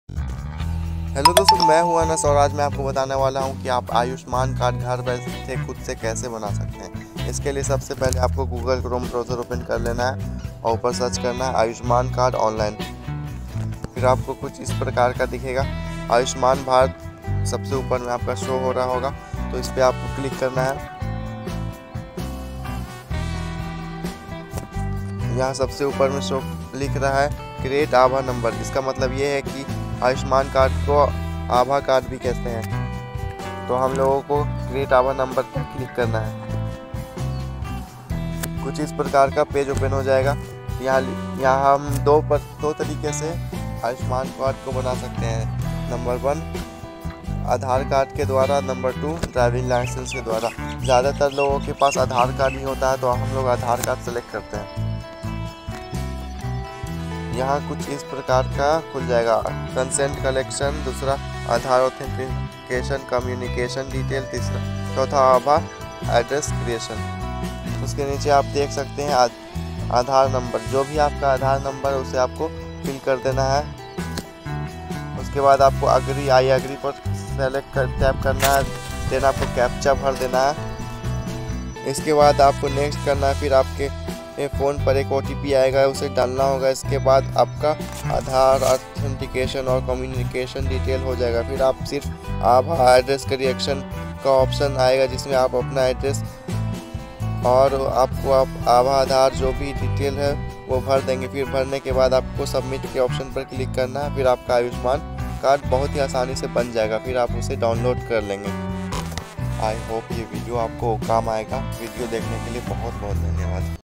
आयुष्मान कार्ड ऐसे बनाए | Ayushman Card से जुड़ी कुछ जानकारी
हैलो दोस्तो मैं हूँ अनस | और आप लोग का स्वागत करता हूँ एक नई वीडियो में | यहाँ मैंने बताया है की आप कैसे कुछ steps ko follow करके ये कार्ड बना सकते हैं |